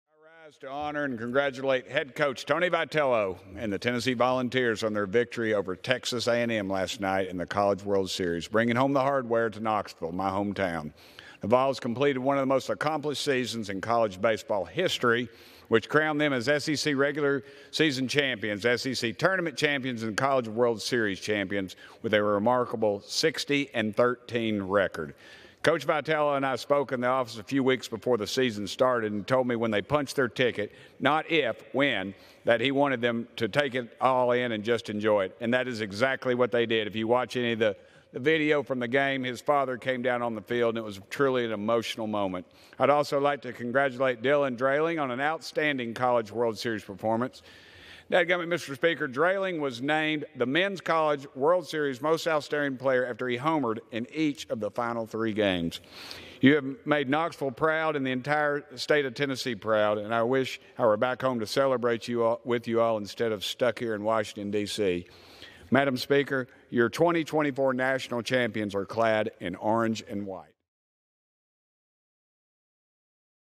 Rep. Burchett gives speech on House floor for Tennessee Volunteers
Congressman Tim Burchett (TN-02) gives a speech on the House floor recognizing the Tennessee Volunteers as the 2024 National Champions.